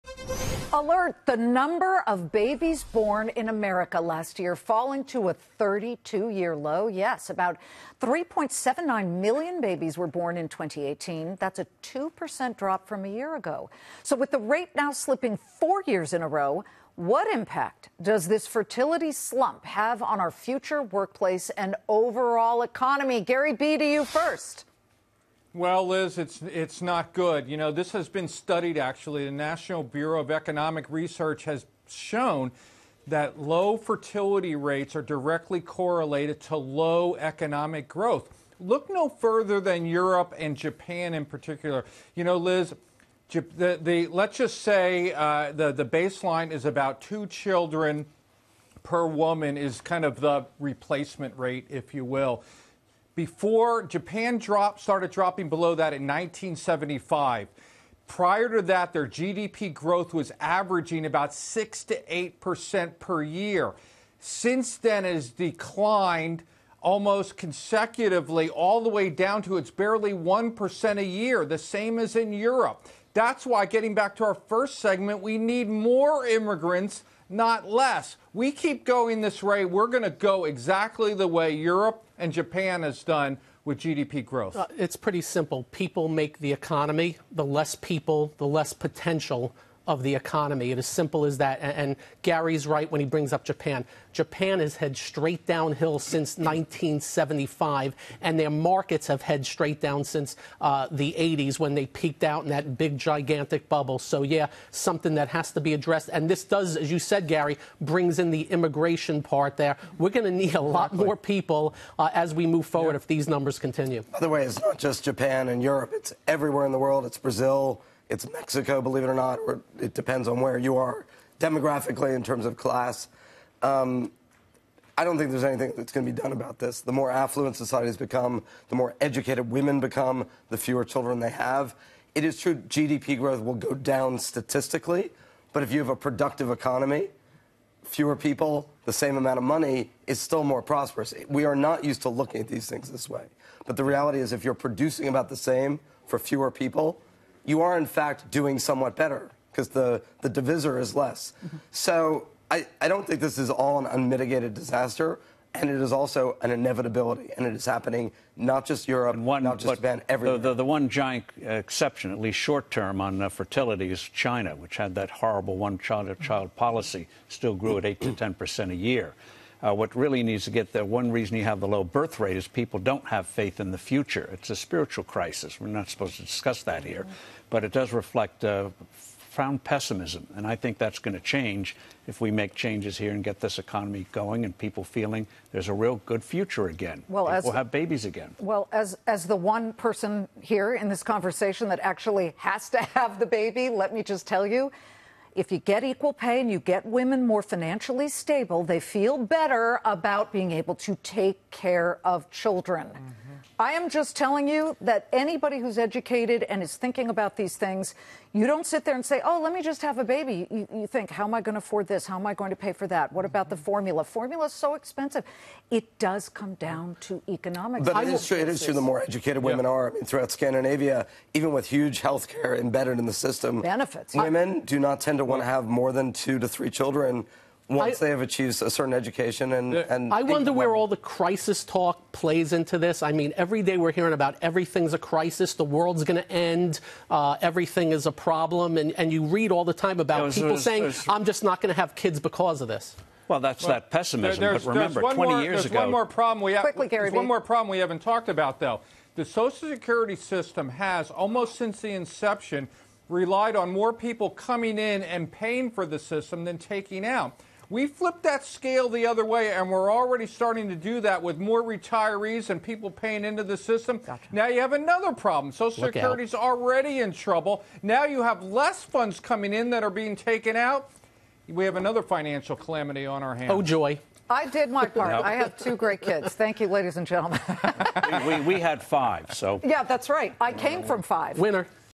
Falling birth rate can lead to an economic decline
Forbes Media Chairman Steve Forbes, Kadina Group’s Gary B. Smith and Kaltbaum Capital Management's Gary Kaltbaum on the potential impact the low fertility rate has the U.S. economy.